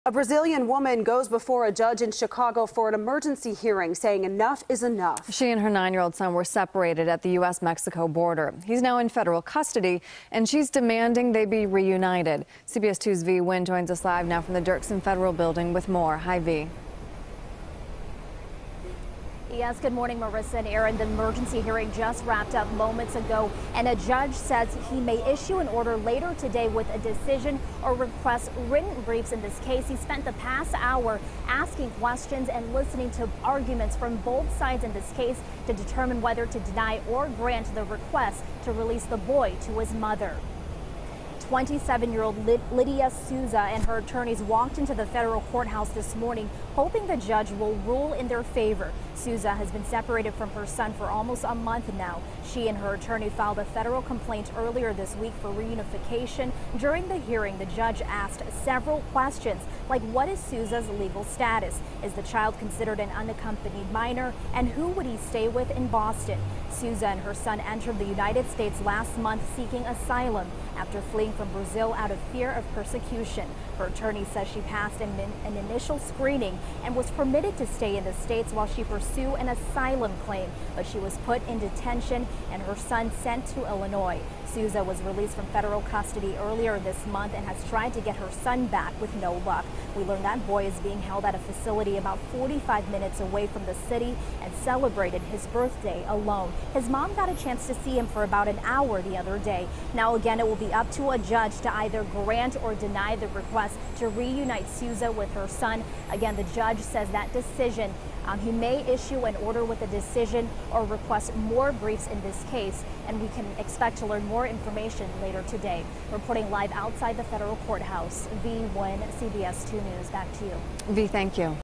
Immigrant Mom Asks Judge To Reunite Her With 9-Year-Old Son
A Brazilian woman separated from her son at the border went before a federal judge Thursday morning, asking for an order to immediately reunite them. CBS 2's VI Nguyen reports.